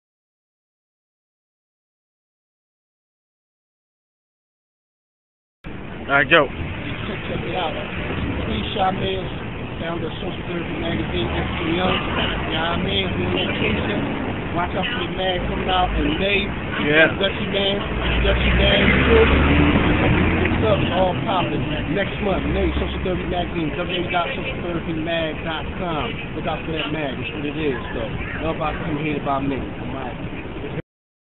DuPree Shamir Founder of Social Therapy Mag speaks on the May issue Ft Dutchie Man
Coming in May to Social Therapy Magazine Feature Artist Dutchie Man